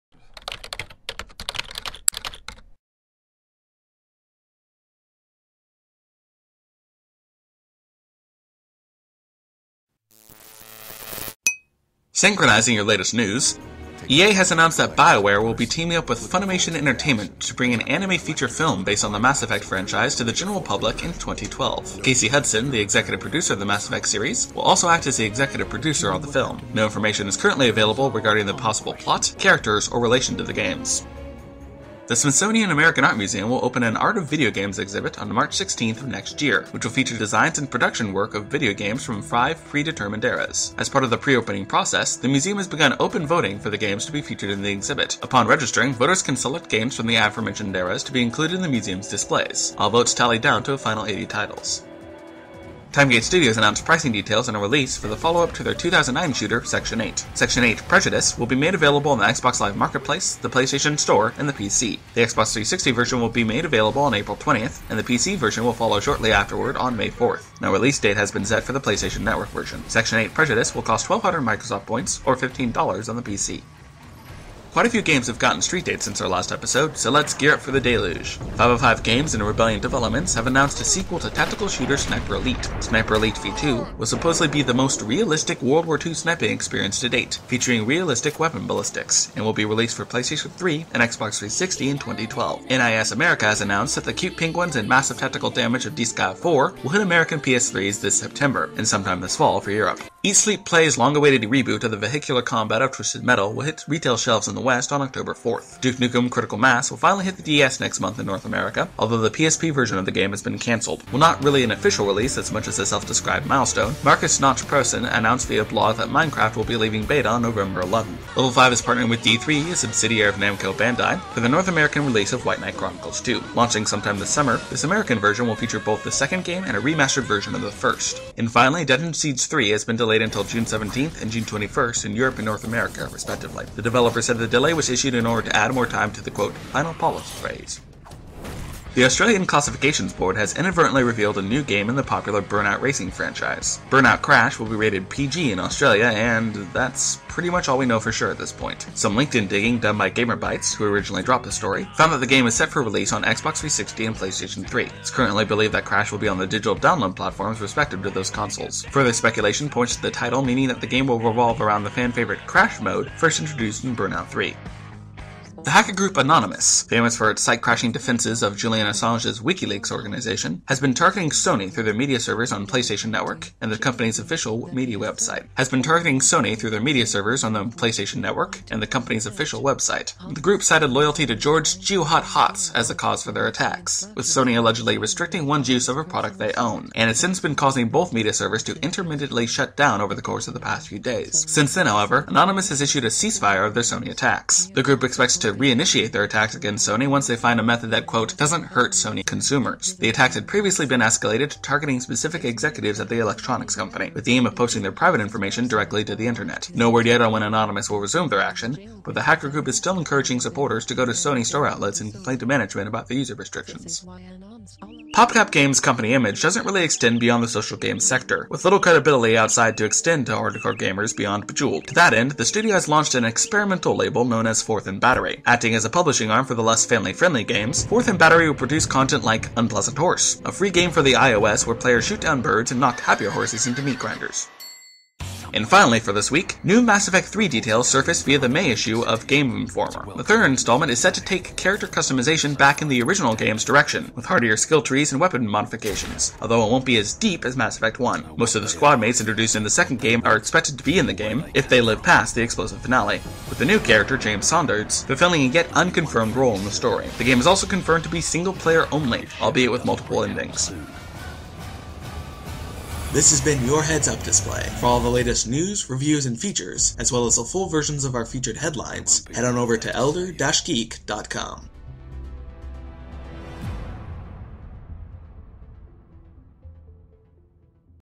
Elder-Geek.com HUD: Episode 5
Heads Up Display or HUD for short is a fast paced news show featuring everything you need to know about the gaming industry, and nothing more.

— Featured News Stories —

Voiceover and writing by Gavin Greene.
Editing and video by Mats Paasche.